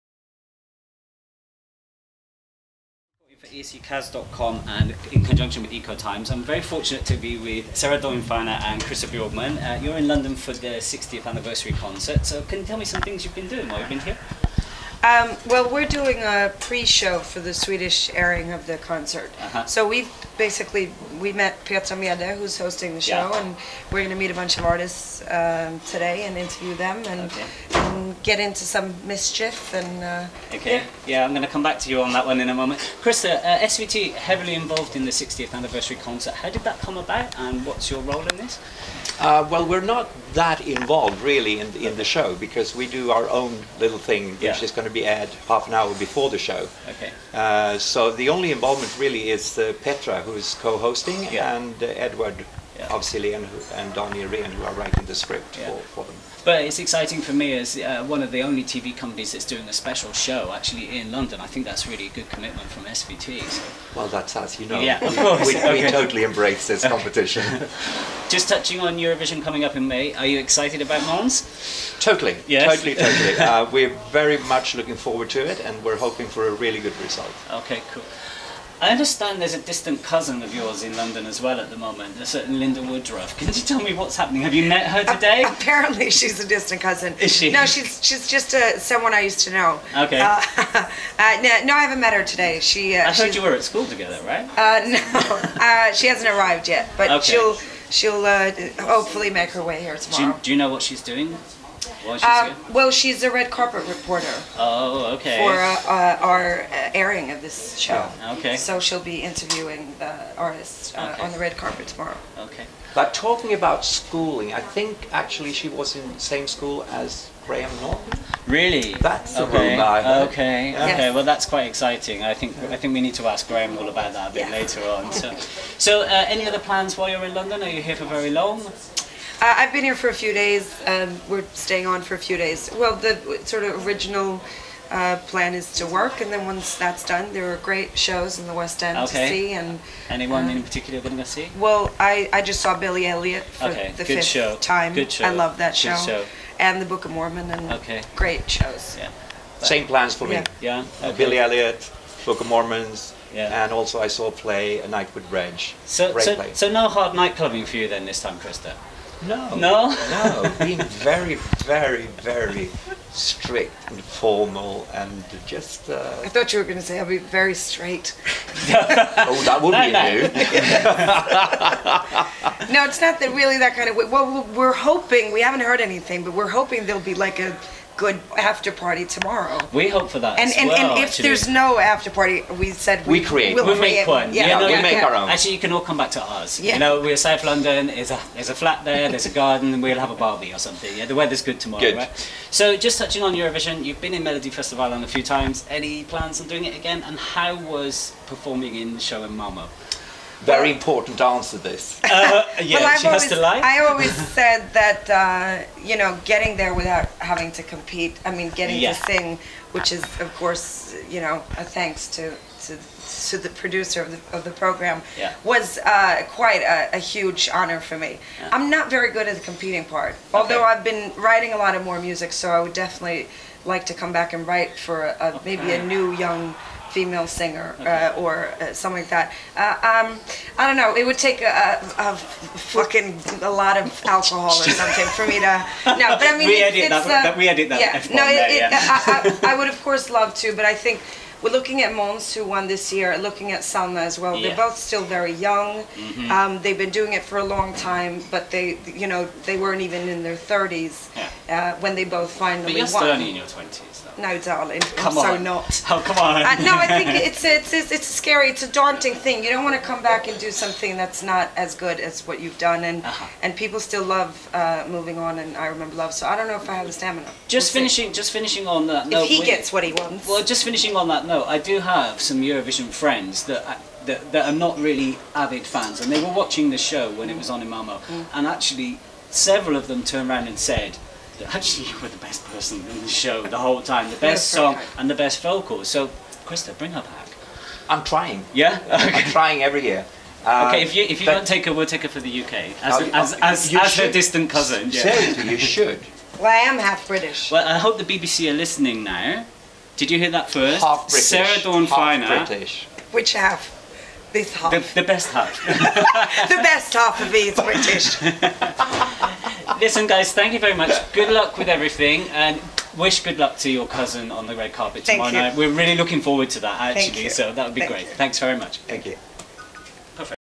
OIKOTIMES: CHRISTER BJÖRKMAN & SARAH DAWN FINER INTERVIEW \ EUROVISION'S GREATEST HITS 2015
LONDON, UNITED KINGDOM - Celebrations for the 60th anniversary of the Eurovision Song Contest are really hitting up as the rehearsals are underway and the final preparations for tomorrow's shows are really gearing up. Today we met Christer Björkman & Sarah Dawn Finer at the SVT HD studio at K West Hotel & Spa where they allowed us to film an interview at their rented studio in the hotel premises. Enjoy the amazing Mr. Melodifestivalen and of course the one and only Lynda Woodruff.